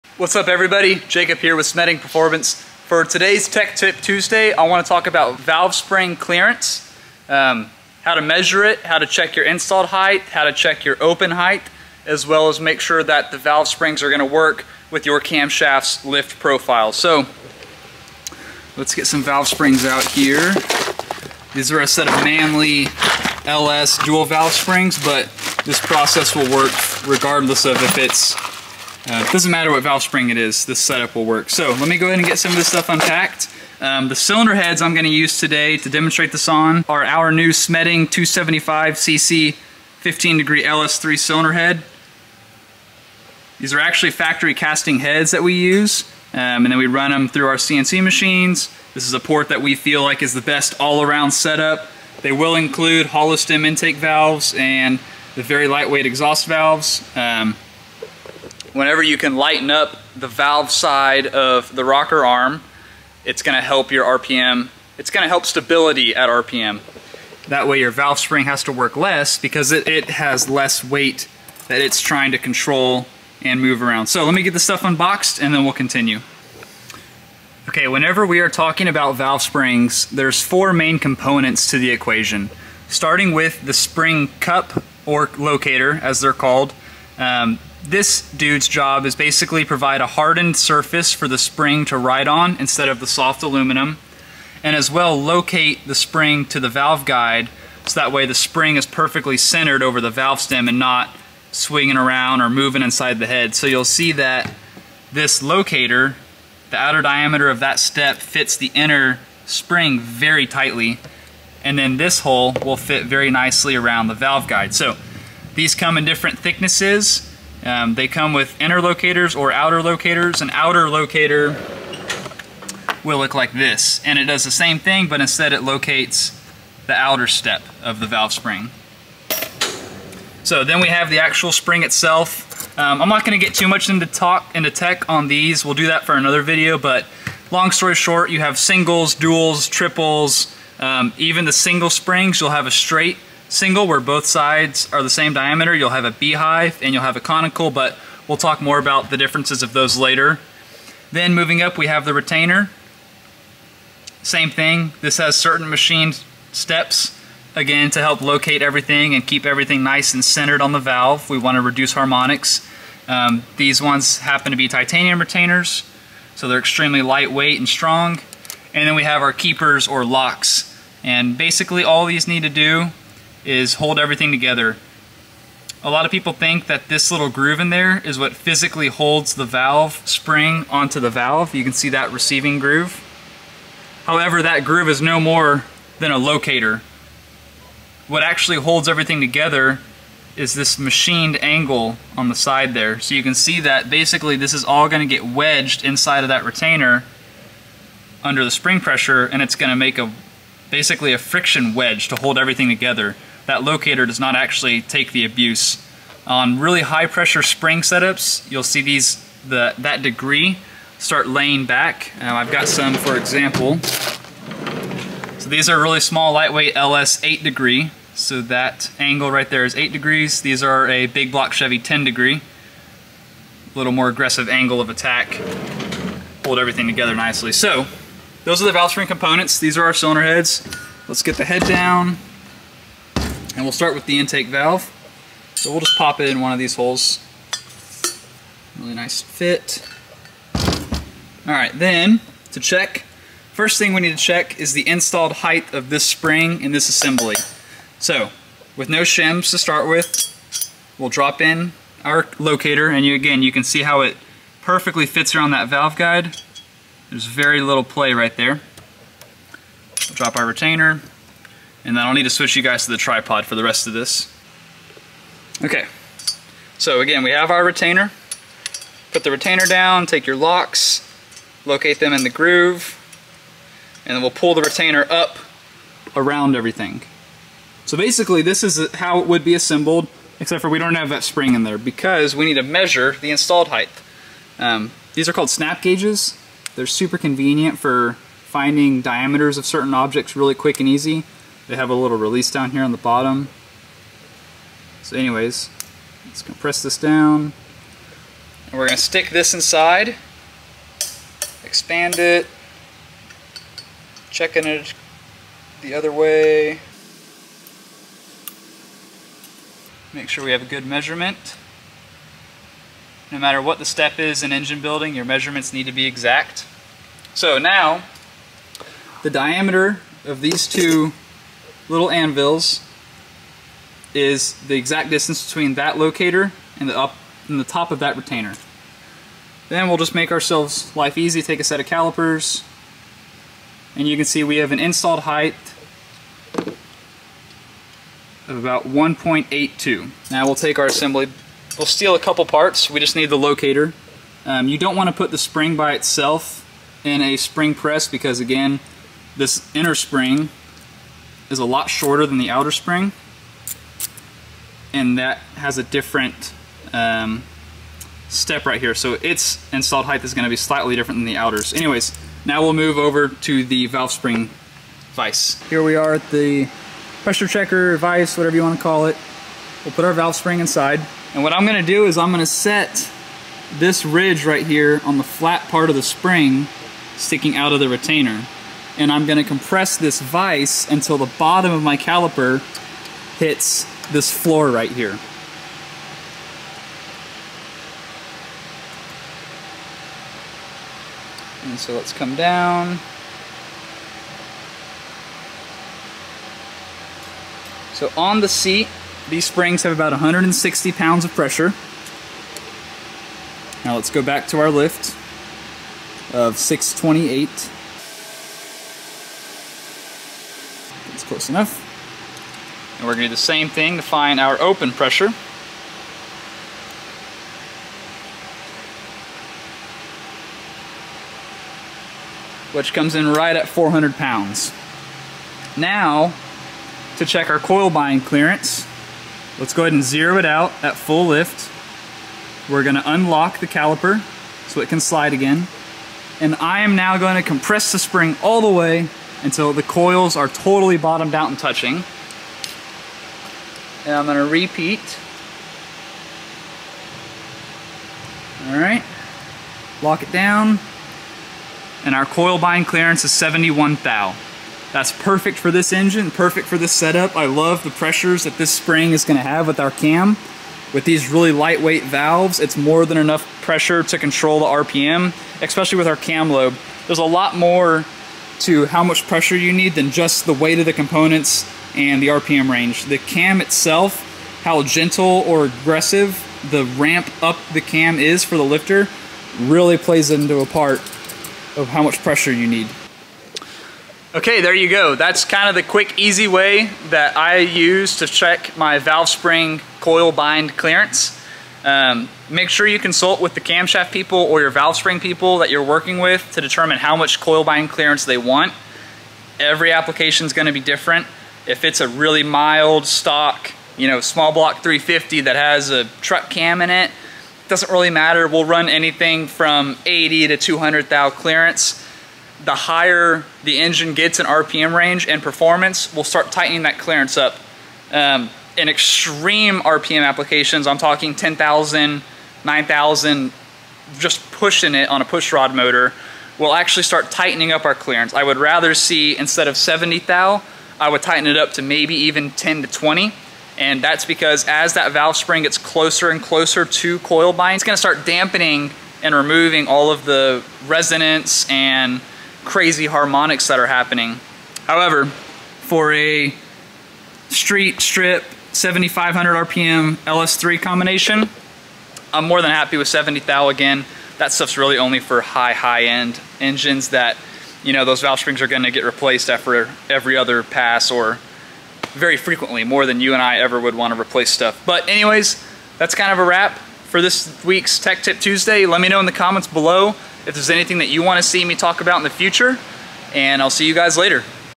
Checking Valve Spring Coil Bind Clearance (correctly) - Tech Tip Tuesday
WHERE I WORK:
MOTOVLOG EQUIPMENT:
Media Mod
Volume Control
GENERAL FILMING EQUIPMENT:
Accessory Kit
Wireless Mic
Lavalier Mic
Micro SD Card